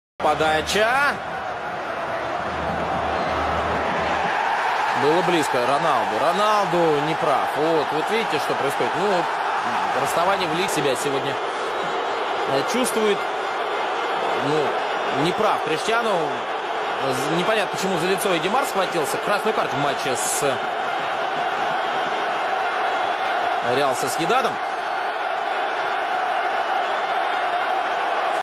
Cristiano Ronaldo- red card vs Cordoba
Have a look at the Rolando's Red card vs Cordoba.